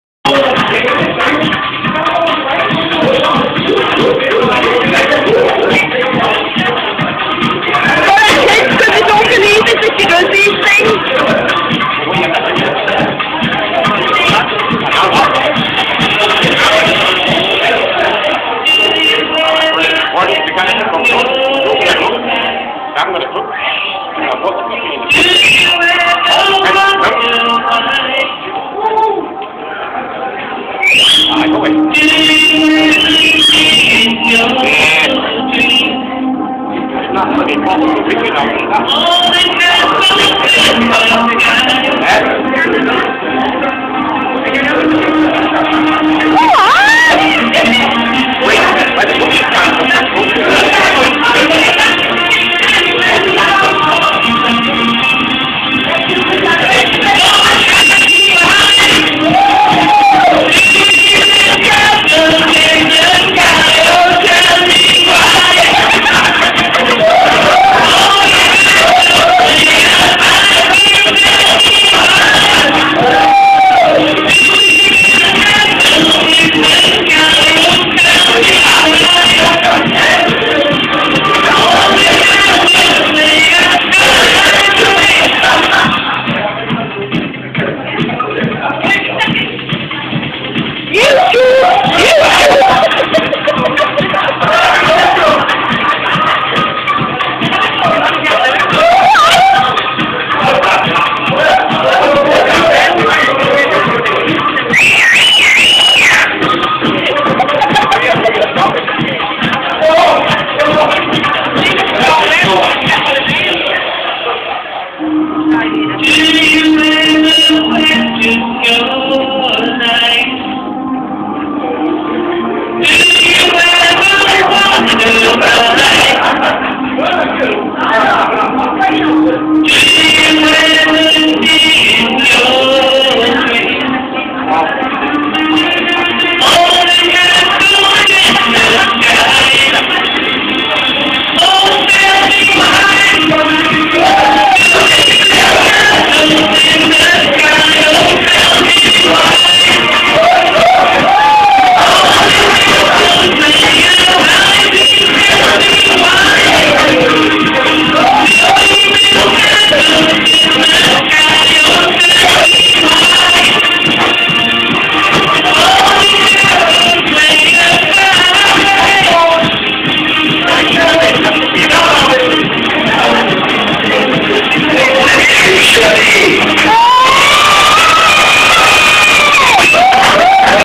shirley karaoke allendale new years eve uk
shirley singing karaoke at the allendale community centre new years eve 2006 byker newcastle upon tyne uk. We had a brill time.